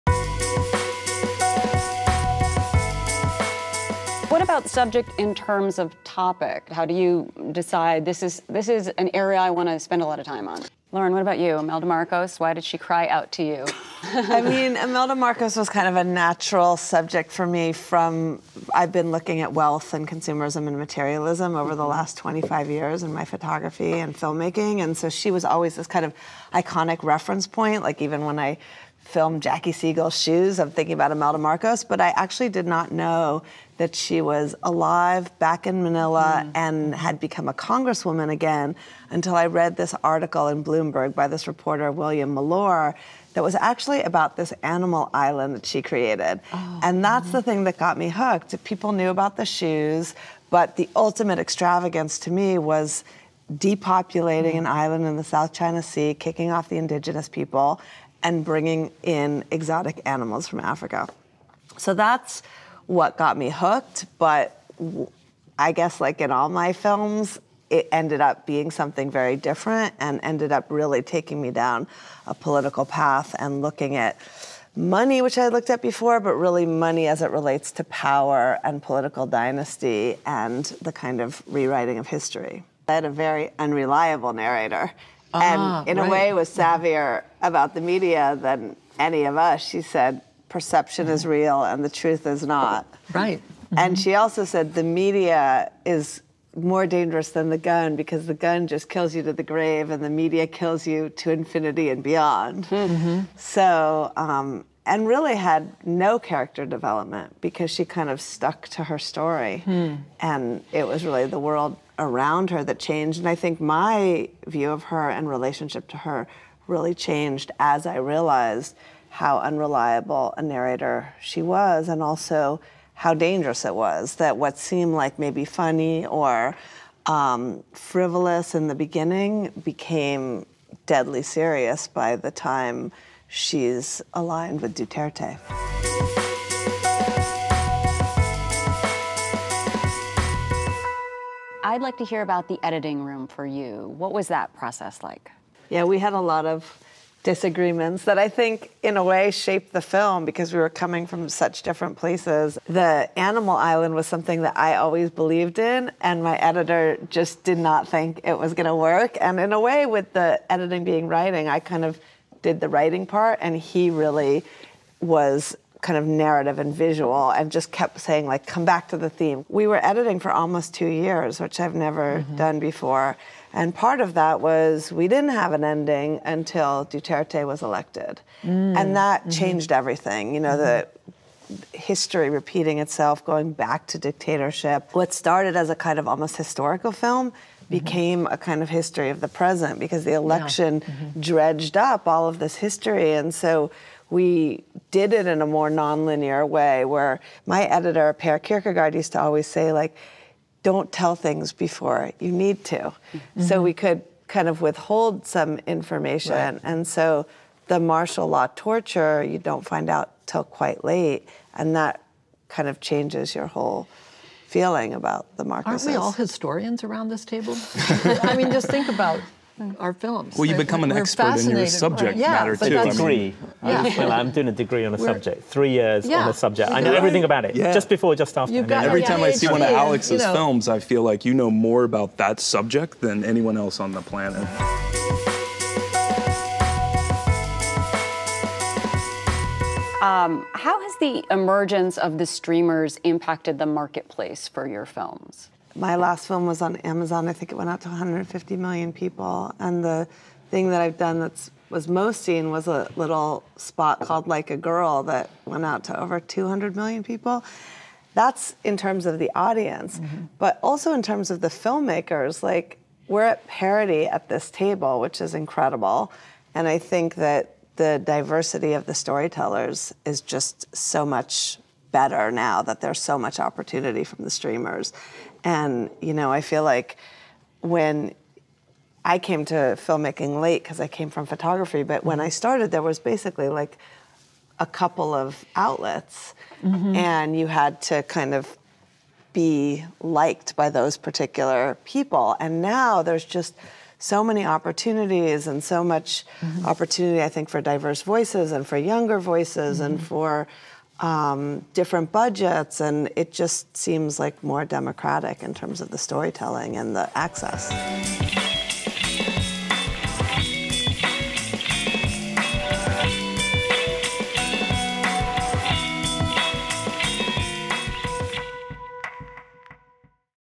Why Imelda Marcos Was a "Natural" Subject for Lauren Greenfield in 'The Kingmaker' | Close Up
Lauren Greenfield ('The Kingmaker') joins Close Up with The Hollywood Reporter for this season's Documentary Roundtable to talk about how her career naturally lead her to focus on Imelda Marcos.